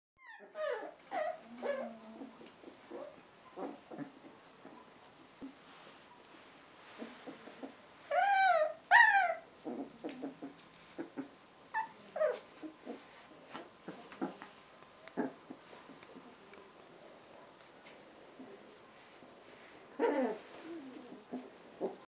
Bambino Di Sole litter "A" (Sealyham Terrier puppies - 2017)
3, 5 weeks old
(Father:: Forlegd Yuppie Moto Moto x Mother: Step Forward Islin)